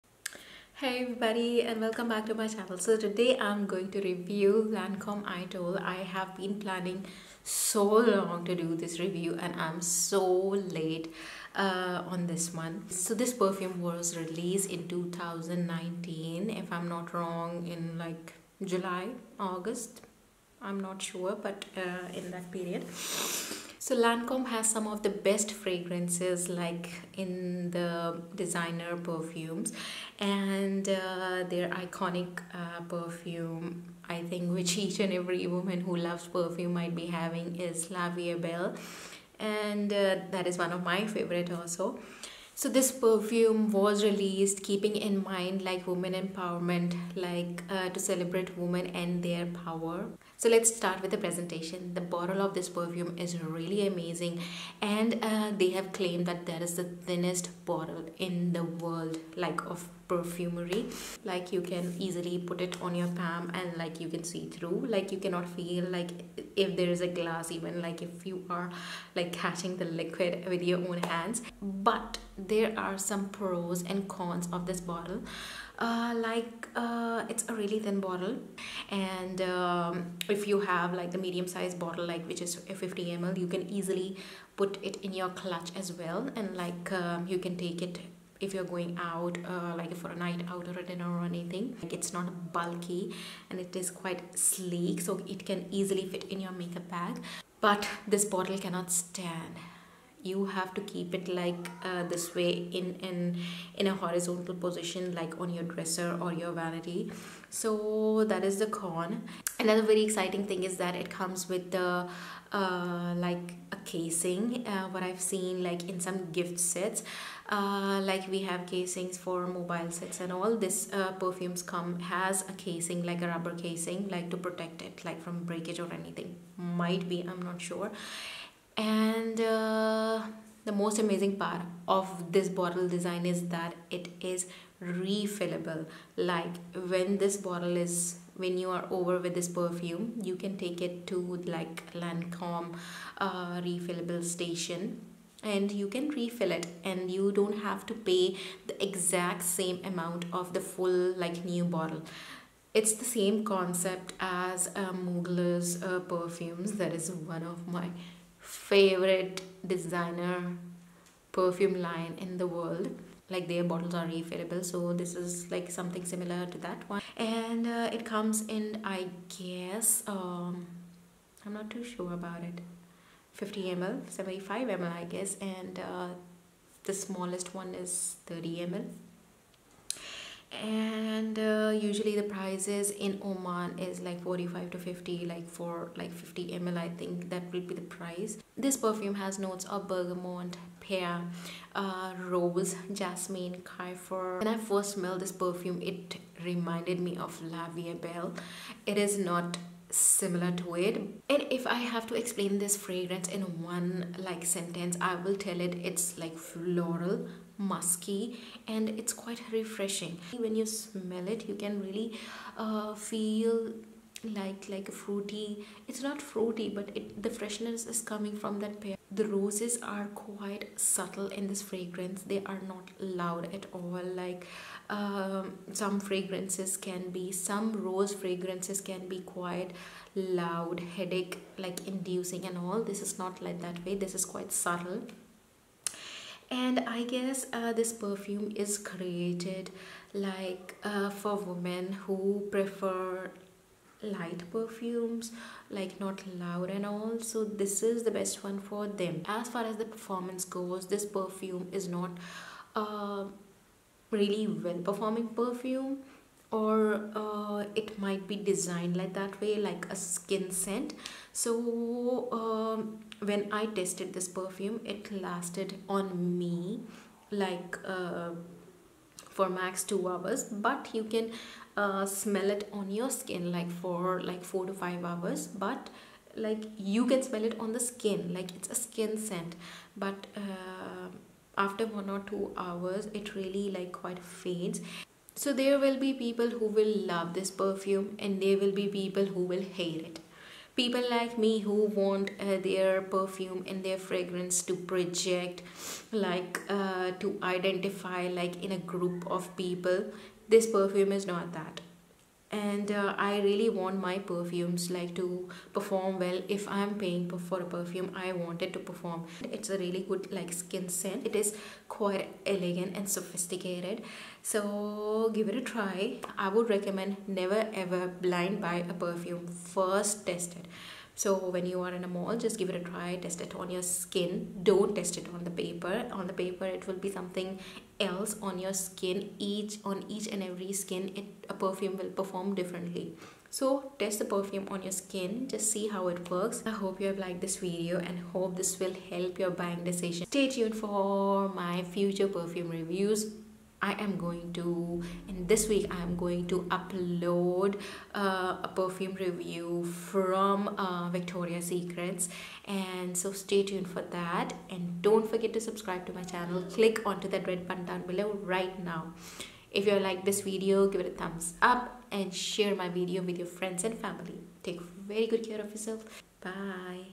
LANCOME IDOLE PERFUME REVIEW - The new Fragrance By Lancome- World's Thinnest perfume bottle "IDOLE"
Finally i have shared my thoughts and reviews on the new fragrance by Lancome which is Idole. This release has so many new and different things from lancome's previous perfume releases. To know my views and thoughts on the new Lancome Idole fragrance watch my full video